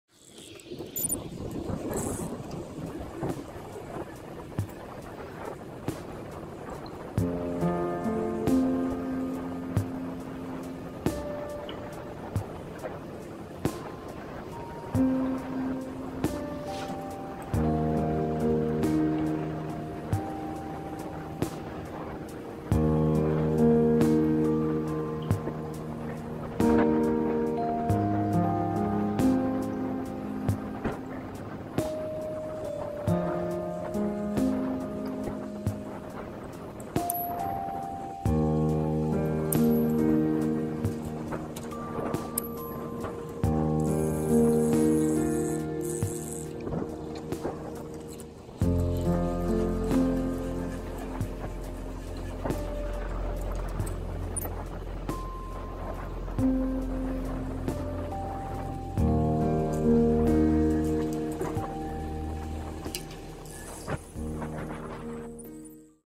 [SO] K70 → Lindental
Mit dem Rennrad von der K70 Richtung Lindental (Geschwindigkeit: 300%). Folgevideos
· Am Ende zurück Richtung K70: demnächst...